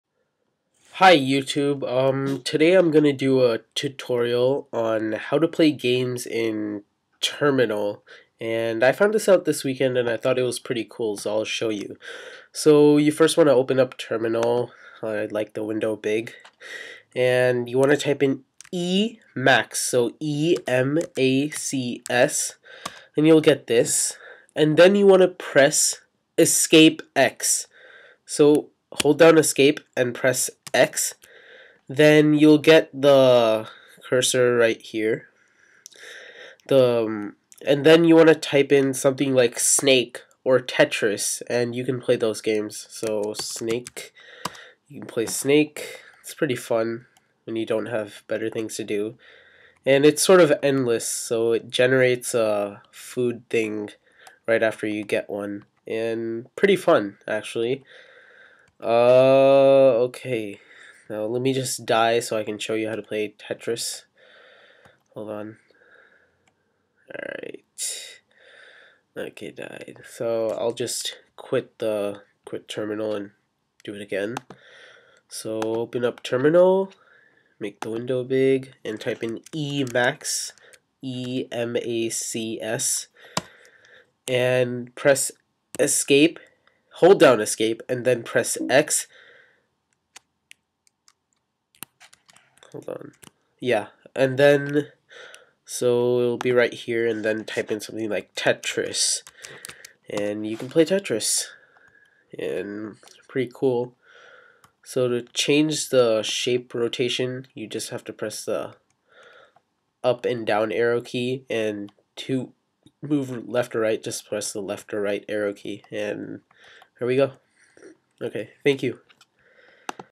How To Play Games in Terminal (Mac OSX)
A tutorial on how to play snake and tetris in mac's terminal program